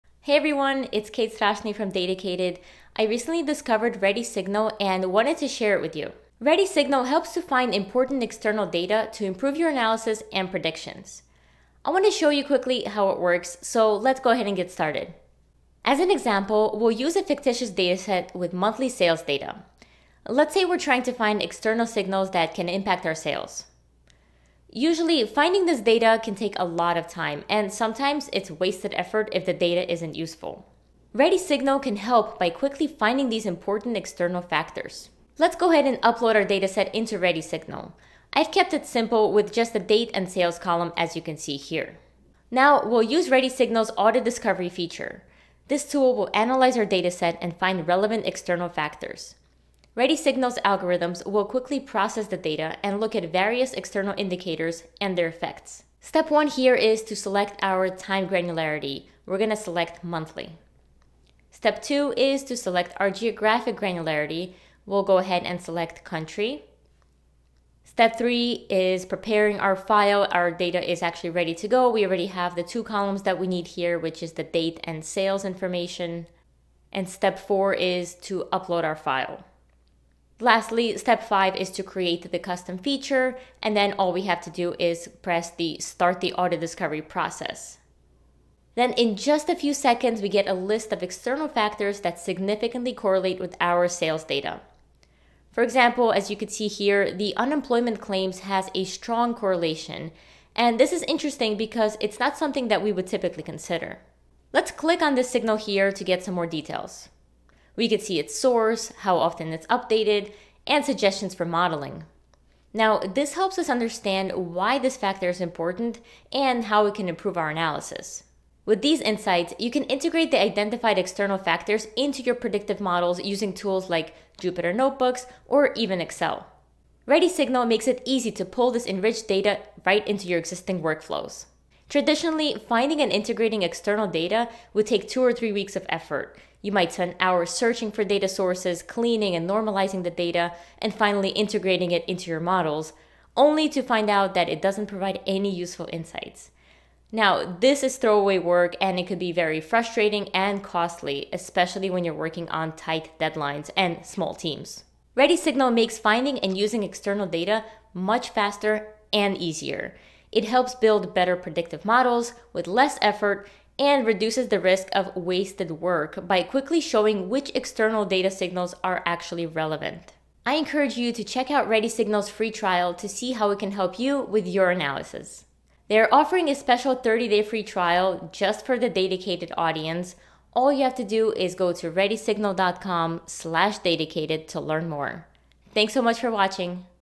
Ready Signal Demo
I'm trying the free trial from Ready Signal to see how I can use 'date' and 'sales' data to uncover external data signals that have strong correlation with my data, as well as some leading indicators.